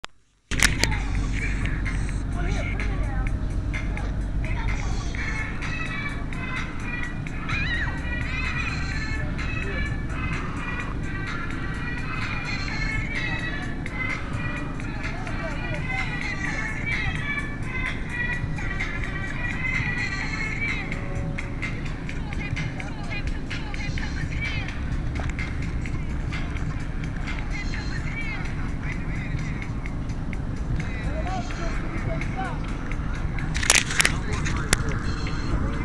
rc10 1.AVI
RC10TEAM 1ST ed.86 or 87  tekin 410g  titanium ti rods stealth trans Iam 1st & only owner of this classic $179.99 for kit only when new... kebob cam